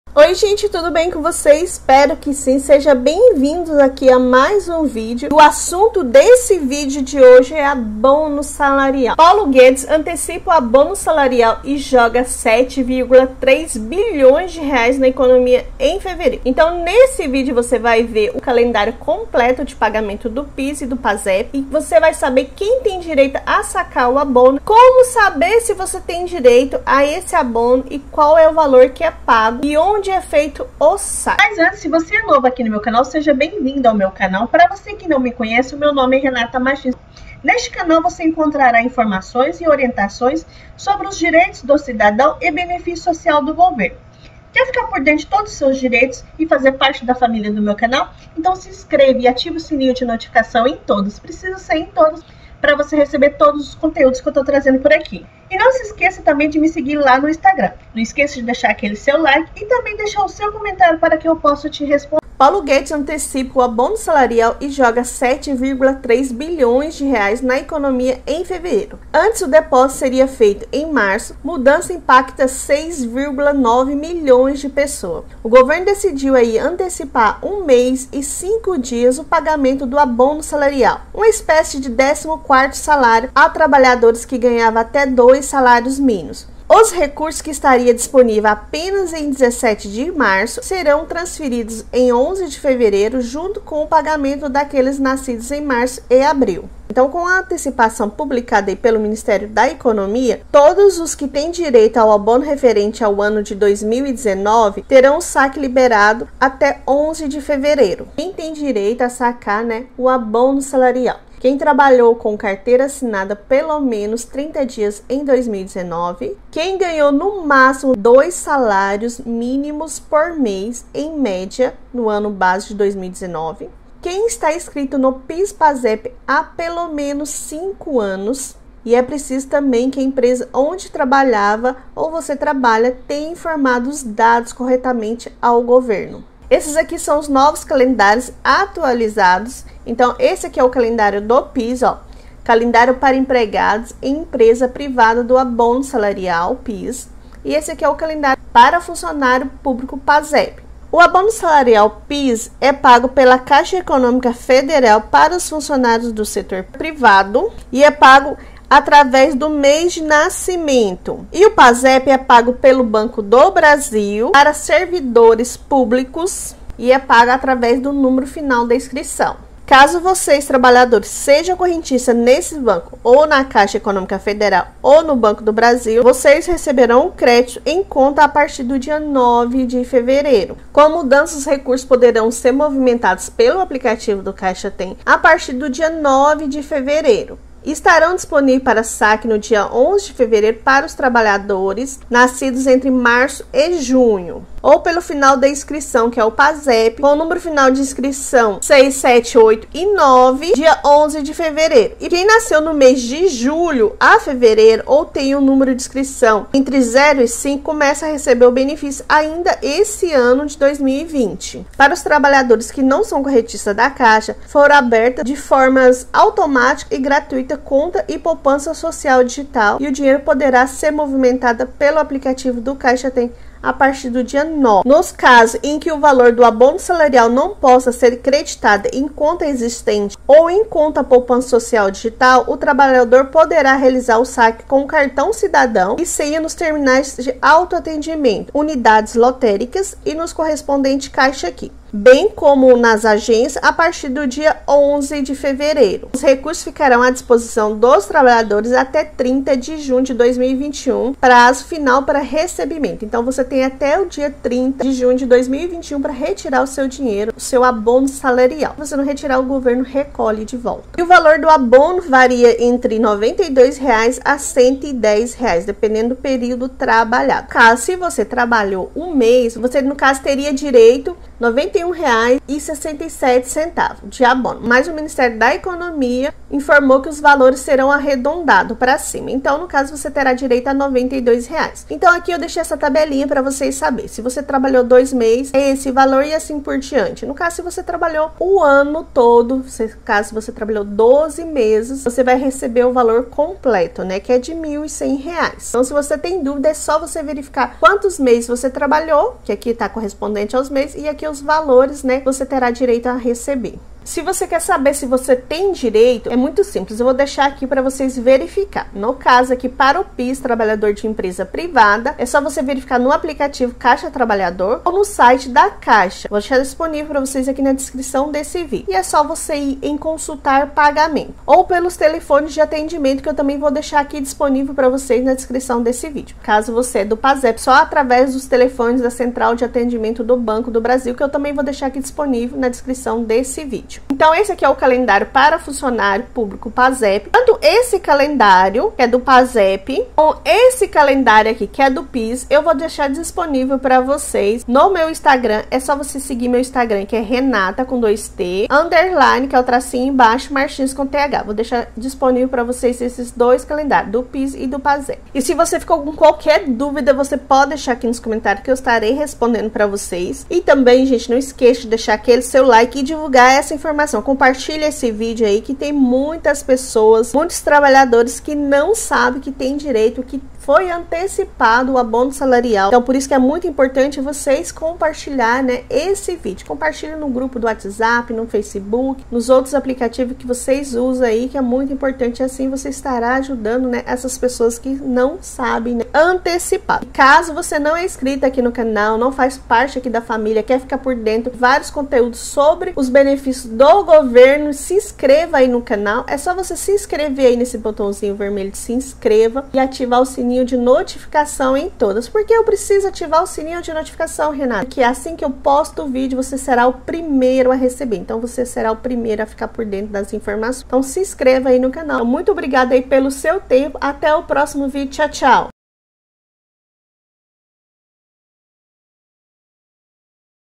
ABONO SALARIAL PAULO GUEDES ANTECIPA PAGAMENTO DO ABONO VEJA NOVO CALENDÁRIO PIS-PASEP 2020-2021
Meus amores, tudo bem? Muito bem-vindos ao canal e a mais um vídeo. Todos os dias trago notícias atualizadas para vocês.

Guedes antecipa abono salarial e joga R$ 7,3 bilhões na economia em fevereiro. Antes, depósitos seriam feitos em março

Como saber se tenho direito? Para saber se tem direito ao abono salarial, é possível fazer a consulta das seguintes maneiras:
PIS (trabalhador de empresa privada):
No Aplicativo Caixa Trabalhador
No site da caixa clique em "Consultar pagamento"
 Pelo telefone de atendimento da Caixa: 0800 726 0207

Pasep (servidor público):
Pelos telefones da central de atendimento do Banco do Brasil: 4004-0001 (capitais e regiões metropolitanas); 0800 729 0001 (demais cidades) e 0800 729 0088 (deficientes auditivos)